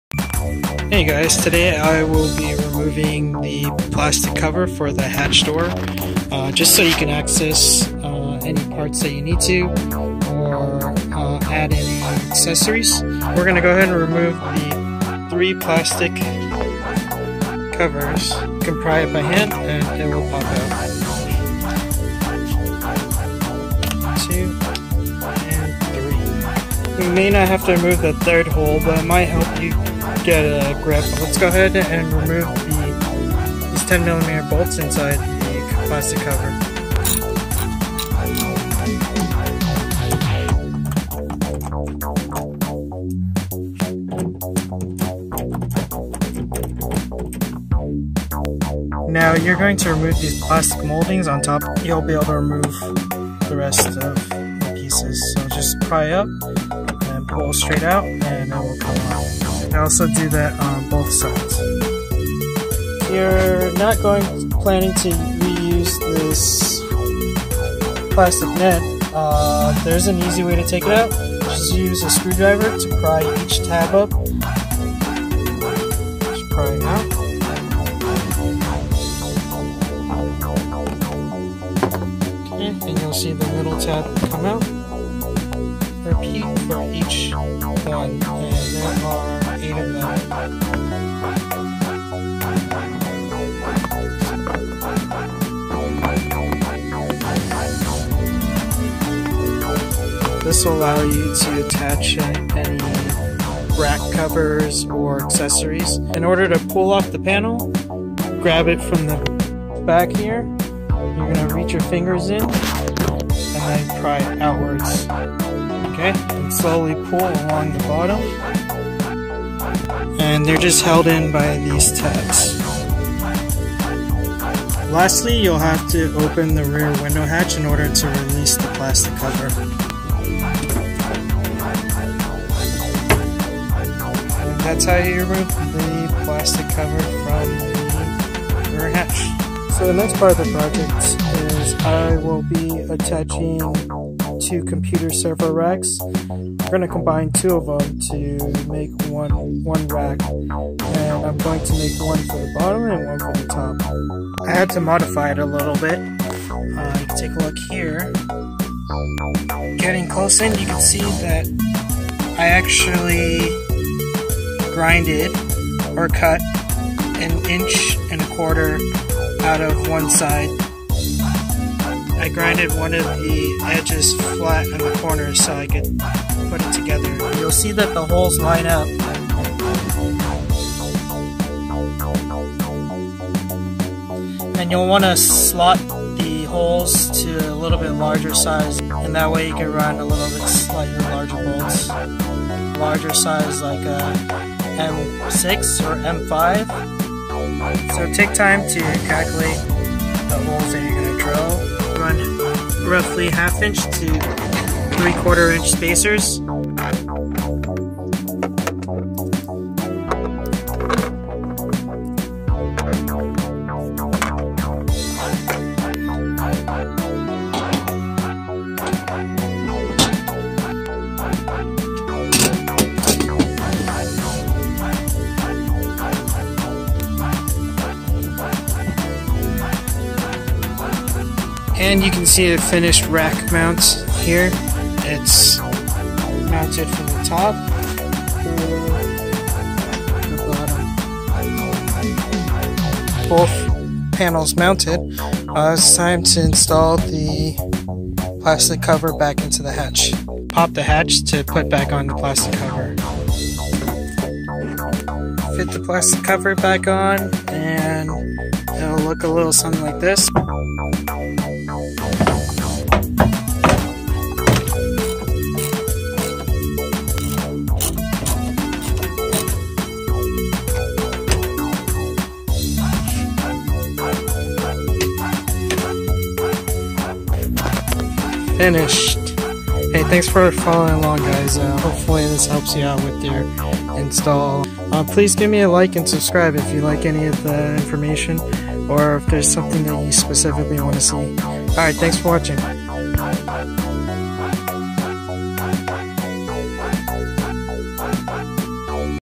DIY: Rear Slotted Rack on your Toyota (FJ Cruiser/4runner/etc)
Example is completed on my FJ Cruiser but with enough  imagination, you can fit one for your Landcruiser, 4runner, RAV4 Highlander, Tacoma, etc for a fraction of the cost.  This allows the use of different mounting solutions such as Velcro straps and magnets.

Thanks to @yellowthunder for sourcing the computer server racks and sharing the info on fjcruiserforums.

Racks used are Navepoint 3U
Larger racks 5U